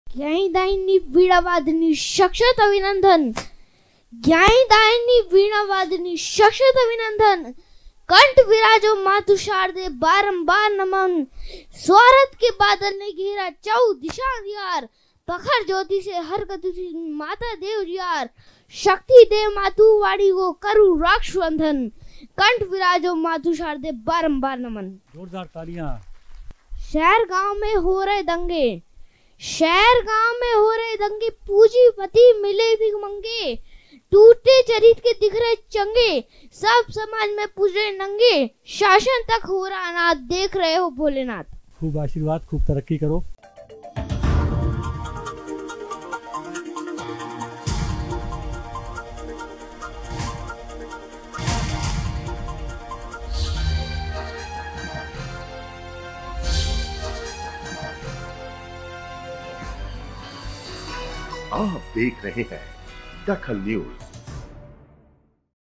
DAKHAL NEWS दख़ल साहित्य में बाल साहित्यकार यशांश दुबे
दख़ल साहित्य में बाल साहित्यकार यशांश दुबे
यशांश दुबे ने रचना पाठ कर सबका मन मोह लिया